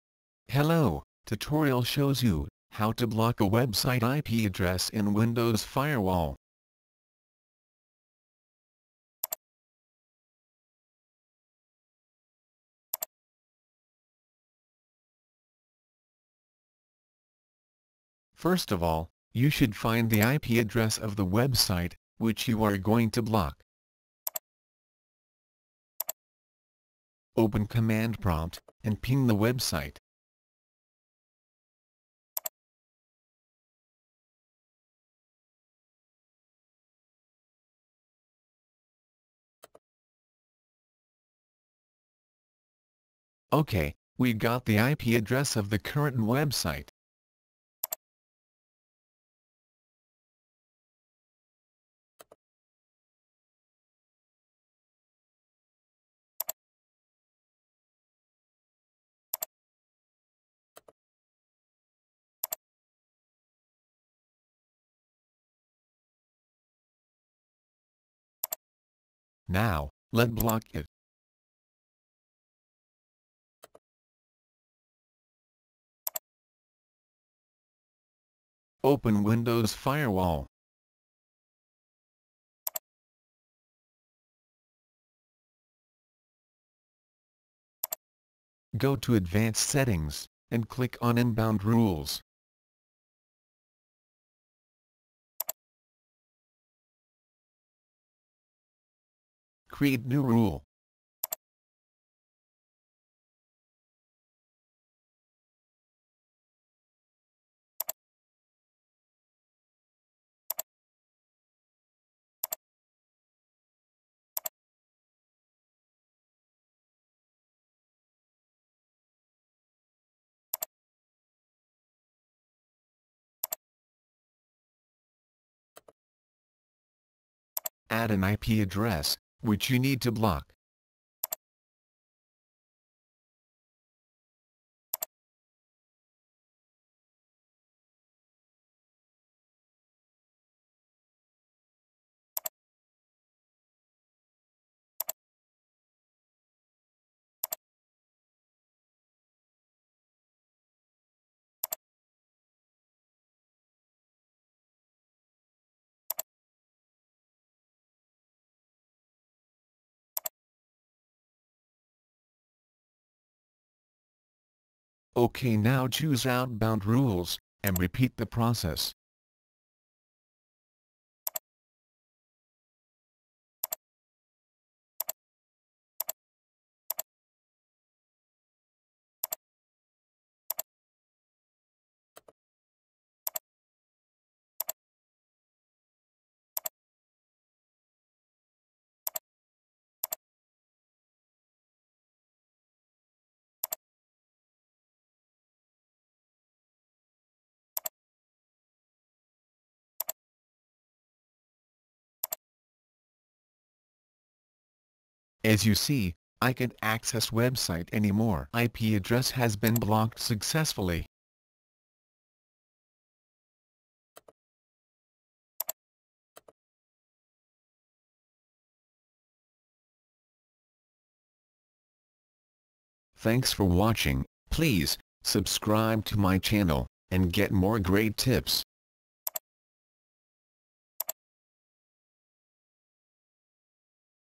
How to block website IP address in windows firewall
Tutorial shows you, how to block website IP address in windows firewall, easiest Way to Block an IP Address.

Blocking website – IP Address with Windows Firewall Steps:
First of all, you should find the IP address of the web site, which you are going to block
Open command prompt and ping the website.
Open Windows Firewall, go to advanced settings
Click on the Inbound Rules tab located on the left side of the window.
After clicking on the Inbound Rules, you will see New Rule tab on the right side of the window.
Click on the New Rule and Choose the Custom Rule which you will find at the end.
Check Custom Rule and click on the Next button.
Next select the All Programs and click Next button.
Click the Next button again.
Select These IP addresses button, which you will find under Remote IP addresses. Click on Add and enter the IP address that you want to block in the designated space.
Click OK and then click Next.
Select Block the connection tab and click on Next to proceed.
The Windows will ask you to write a description for the firewall rule. After entering the description, click on Next button.
Click on Finish to create and implement this rule that will block the desired IP address
Final step would be to choose Outbound Rules on the left side of the window and repeat the process.